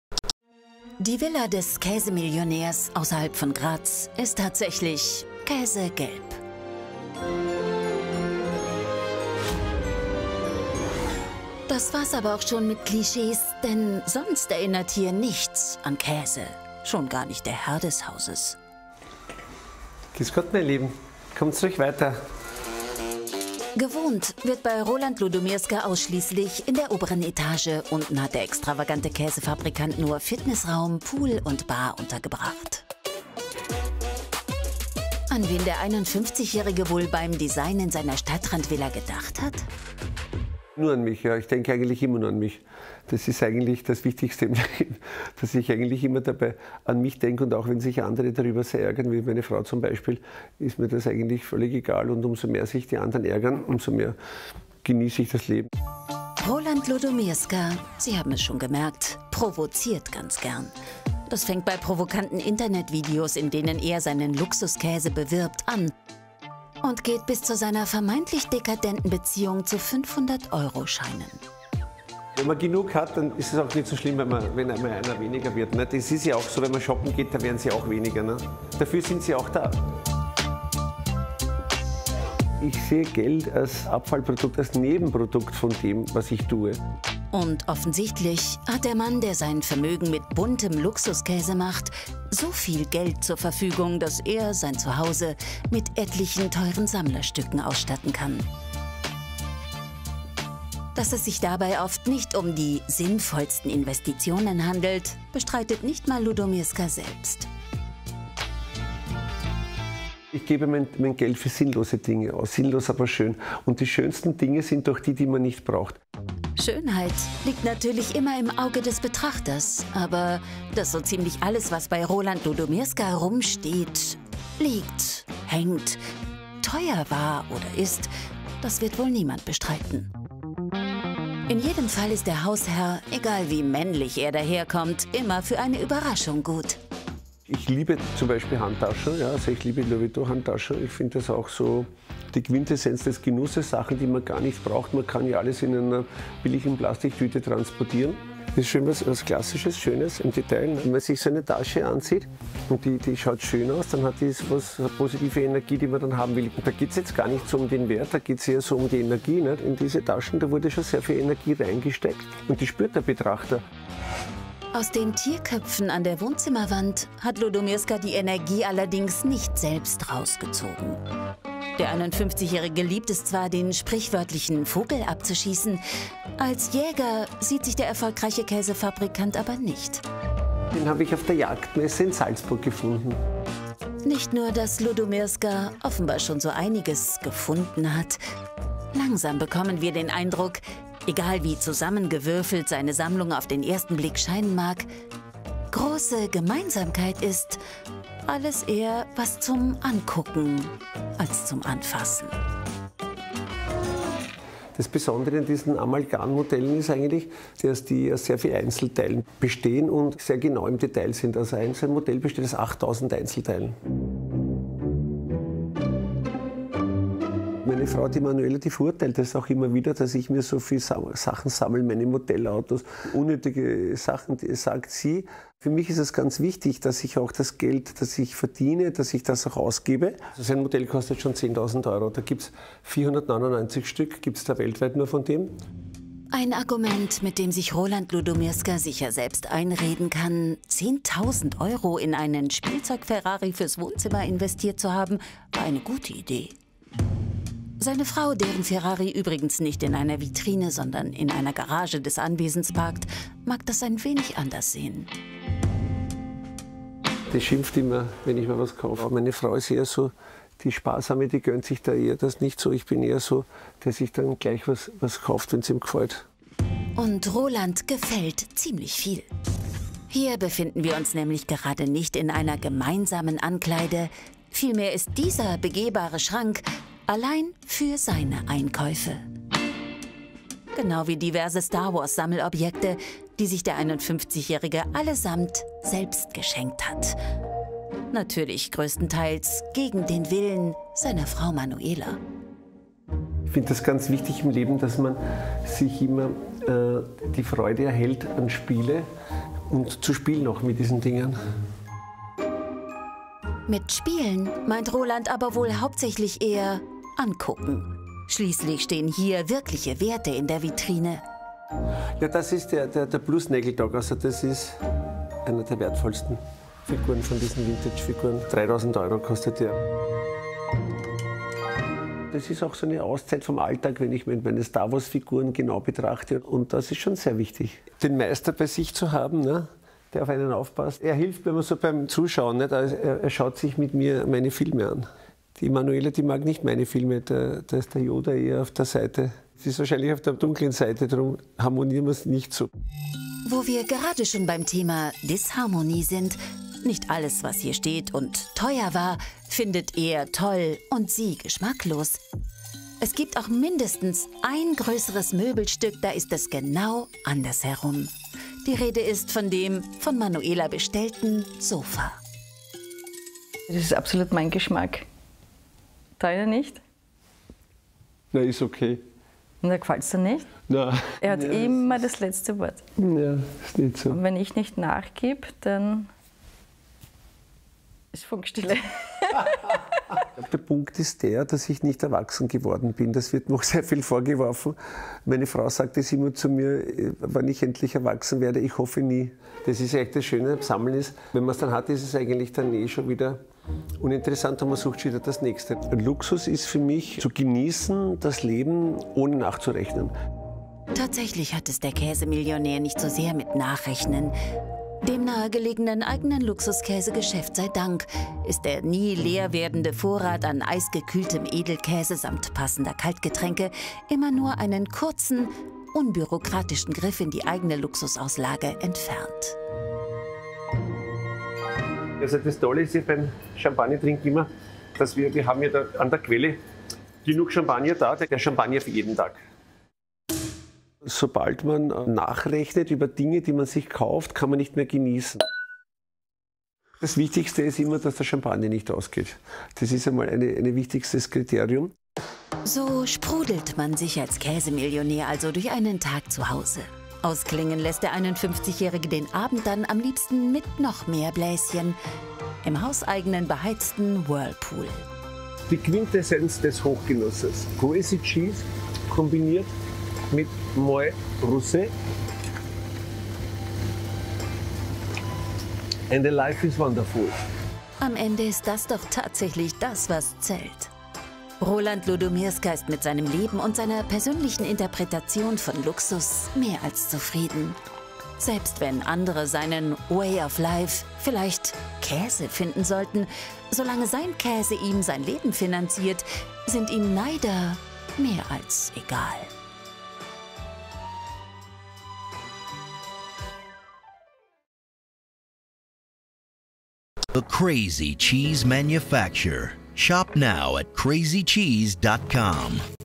Crazy Cheese TV NOW Beitrag 📺🧀
Letzte Woche hatte ich das Fernsehteam von TVNOW bei mir zu Hause. Den ganzen Beitrag könnt Ihr hier nachsehen, meine Lieben! 📺👌🏻